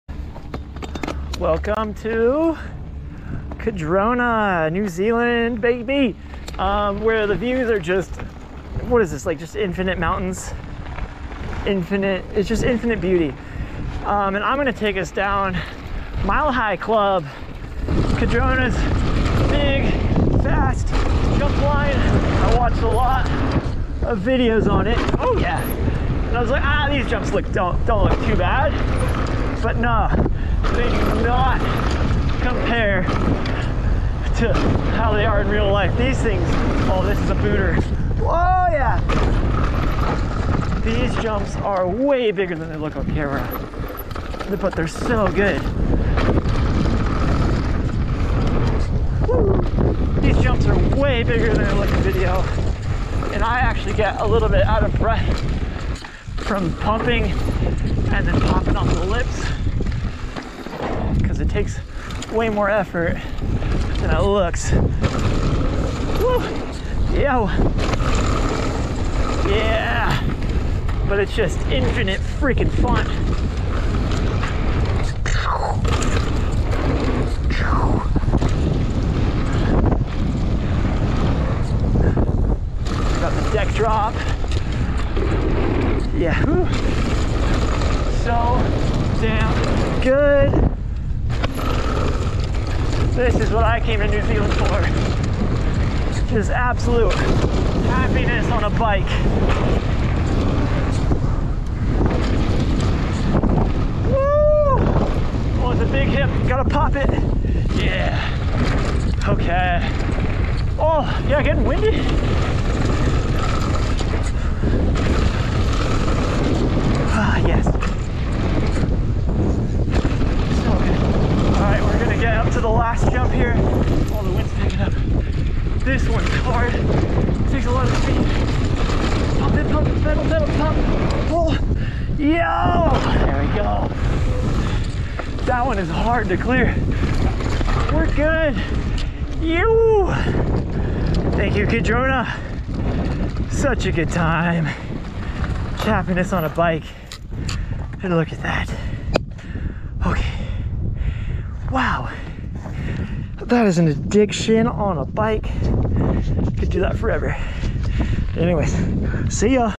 Cardrona - Mile High Club
Cardrona Bike Park!! This place has been highest on my list of dream destinations, but I never thought it would be possible to actually make it. It's in New Zealand, on a remote mountain, and up a sketchy gravel road. But holy freaking moly, if you chase your dreams I guess they actually do come true